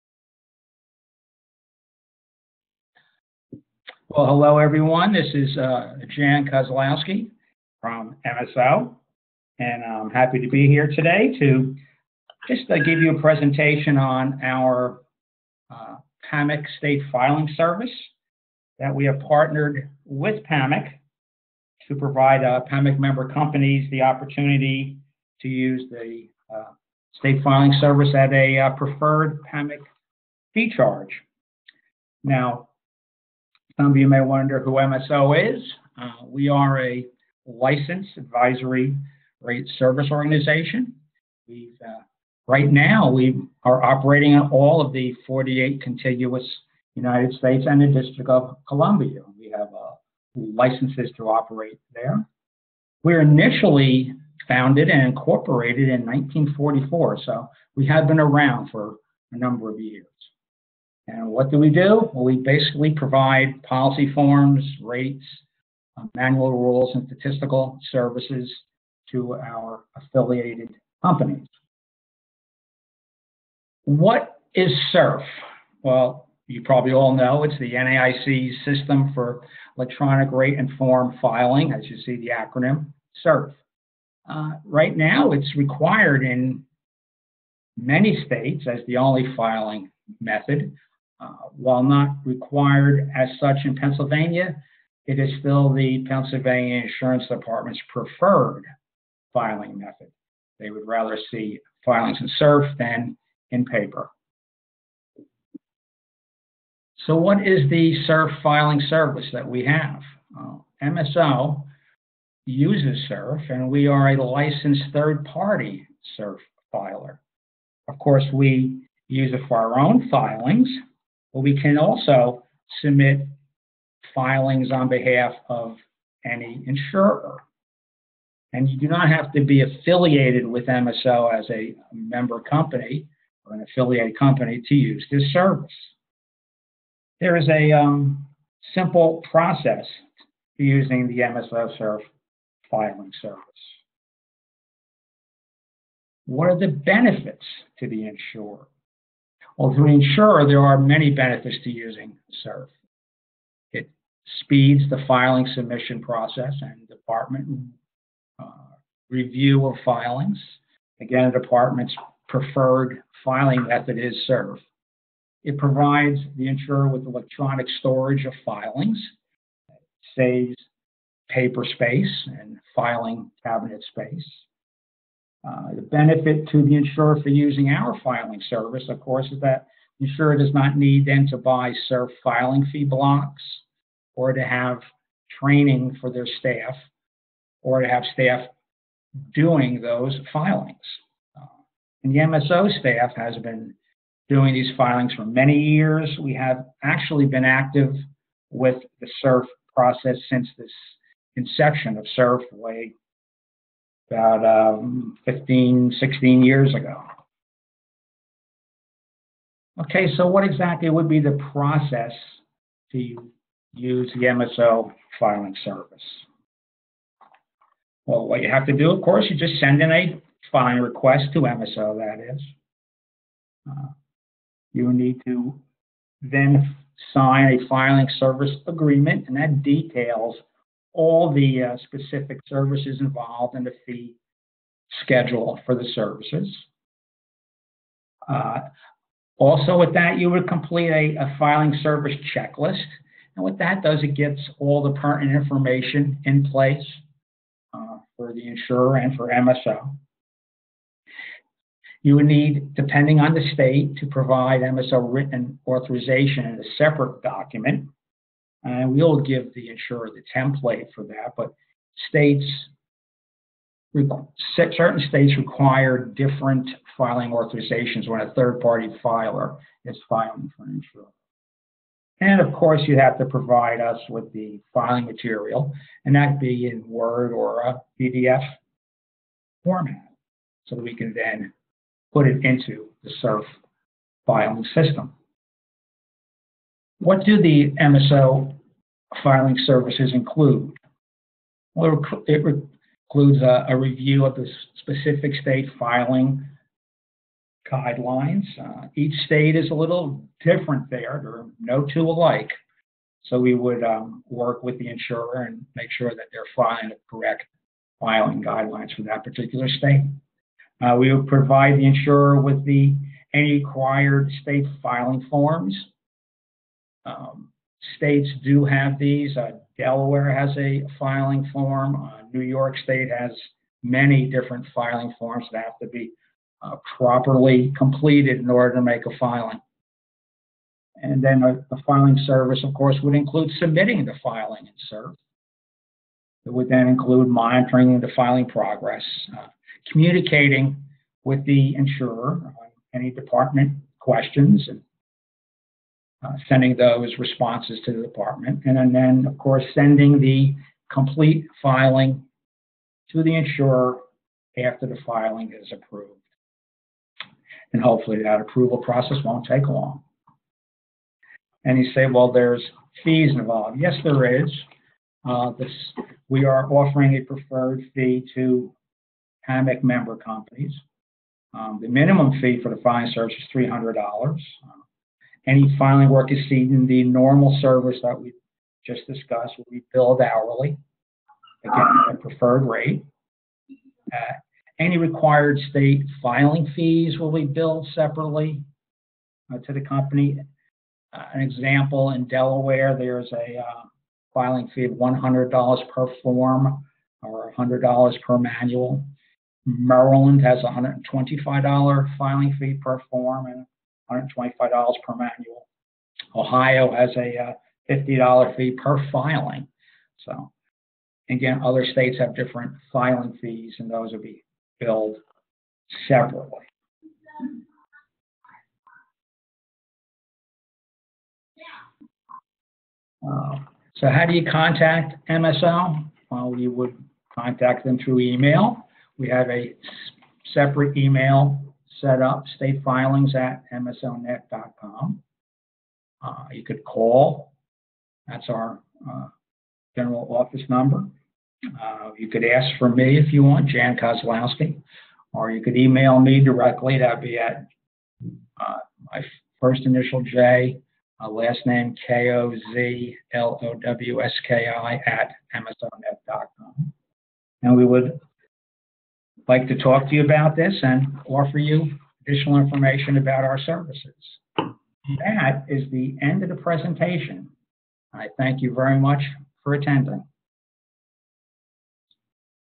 Member Benefit: MSO SERFF Filing
MSO has partnered with PAMIC to offer discounted SERFF Filings to our membership.